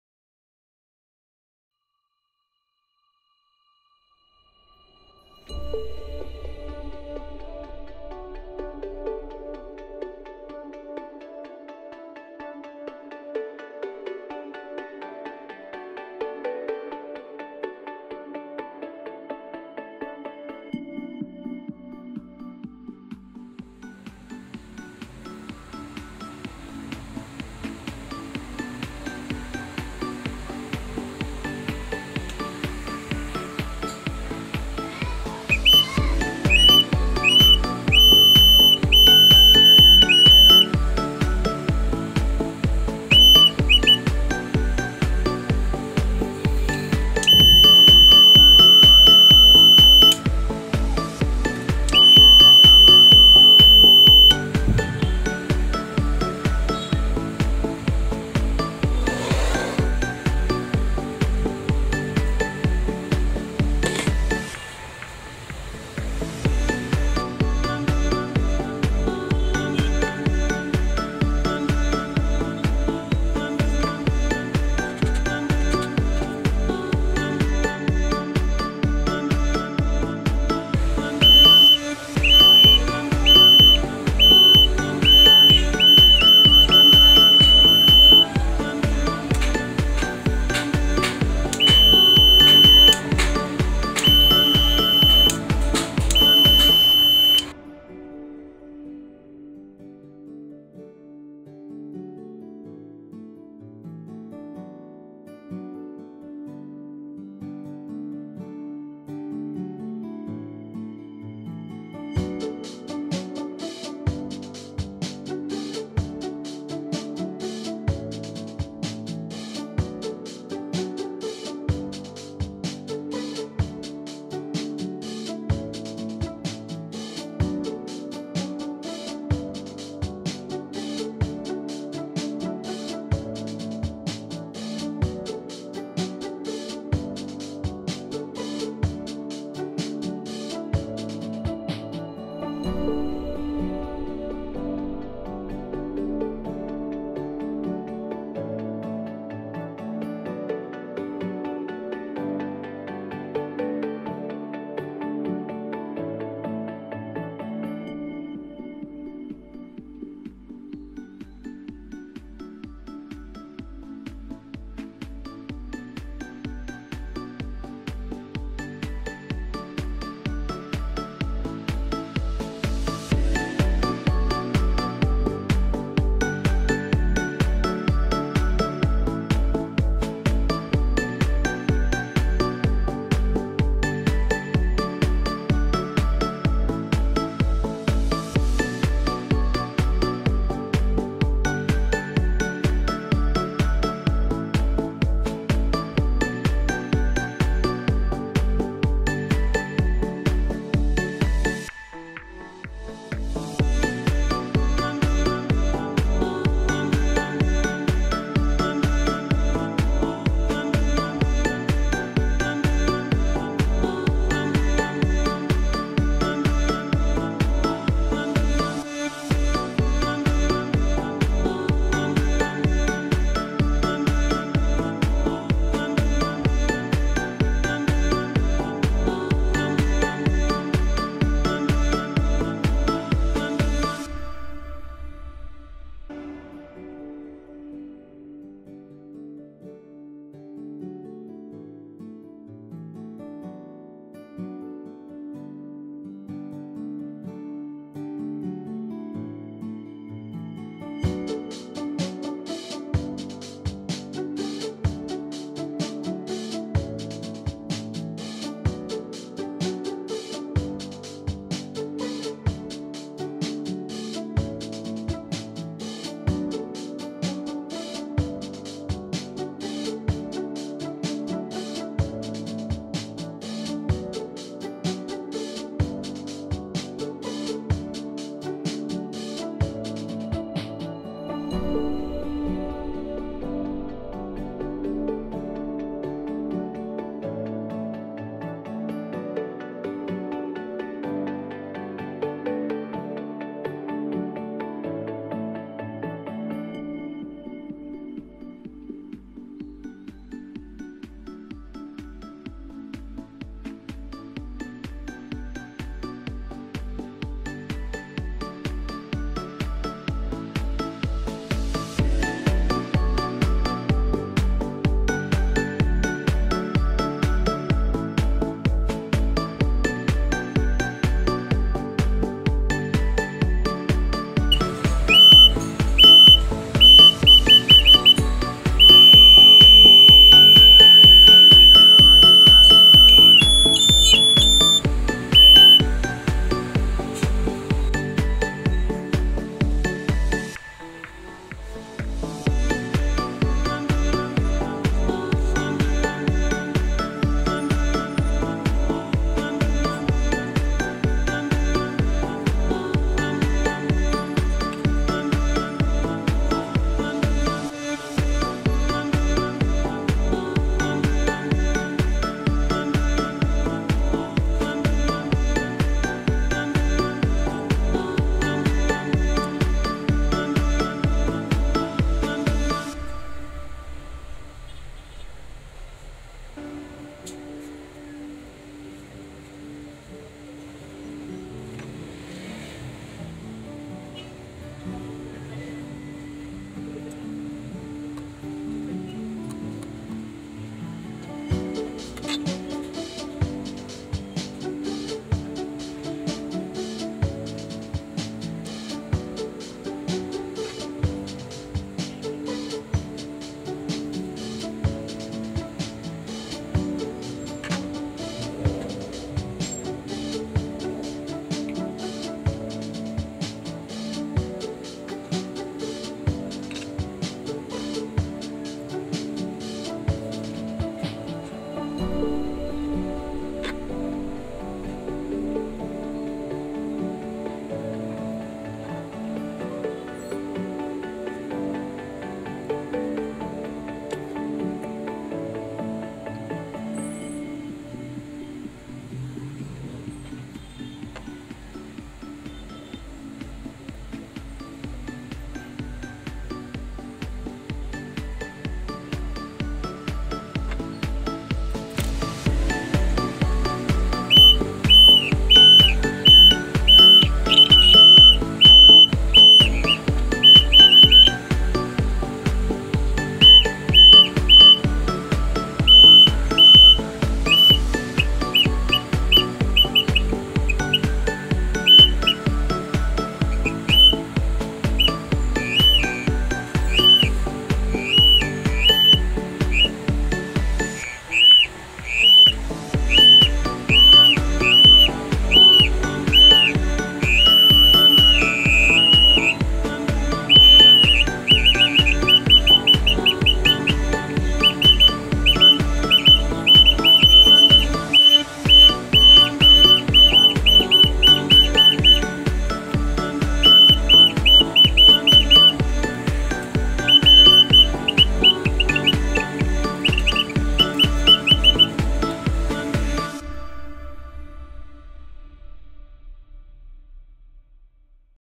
How to Make a LDR Darkness Sensor Buzzer with transistor BC547 - Breadboard
This video will show you how to make a dark sensor buzzer, where when darkness exists the buzzer will make a noise indicating that it is dark in the surrounding. The function of the circuit was possible because of the photoresistor and transistor.

Materials:
9V Battery
Resistor 100k ohms
Buzzer
Photoresistor
Transistor BC547